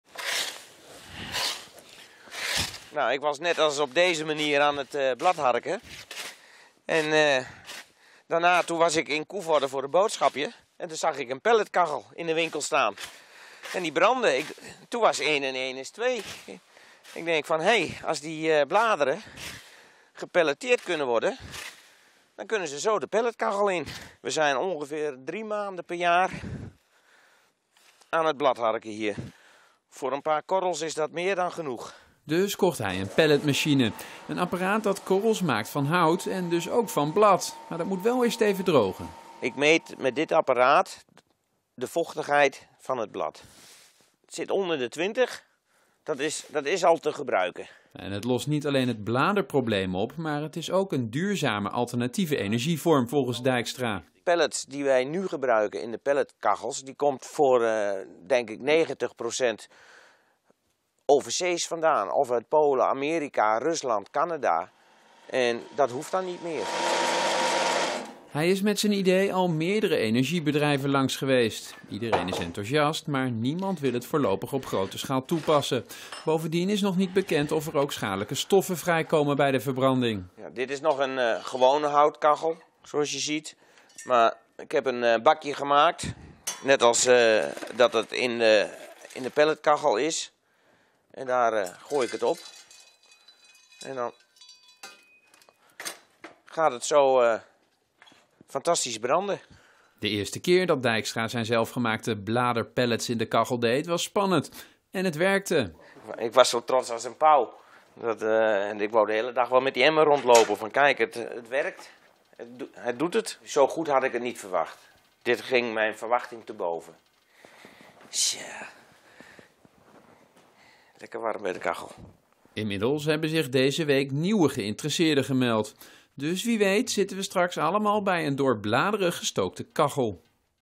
Inwoner van Gees laat kachel branden op bladafval
GEES - Freek Dijkstra uit Gees maakt van de bladeren in zijn tuin korrels waarmee hij de kachel kan laten branden. Hij hoopt dat anderen zijn idee overnemen.

Nu worden bladeren gezien als onhandig afval. Gemeenten moeten betalen voor de verwerking ervan. Het wordt gecomposteerd. Volgens Dijkstra is het beter om de bladeren te verwerken tot zogenoemde pellets. Hij bedacht het plan tijdens het bladharken. Kocht een pelletmachine en de korrels bleken uitstekend te branden. "Ik was zo trots als een pauw!"

Enthousiast
Met zijn idee is hij langs energiemaatschappijen gegaan. Die waren enthousiast, maar zagen niets in een grootschalige toepassing. Inmiddels heeft Dijkstra nieuwe contacten gelegd met onder andere de provincie.

Schone energie?
Hoe schoon het verbranden van bladeren precies is, weet niemand. Er is nog geen onderzoek naar gedaan. Dijkstra wijst erop dat pellets gemaakt van hout nu vaak uit andere landen worden geïmporteerd. Dat is ook slecht voor het milieu.

Inmiddels brengen verschillende dorpsgenoten hun bladeren al bij hem langs. Deze winter kan hij de kachel laten branden op zijn eigengemaakte en bedachte korrels.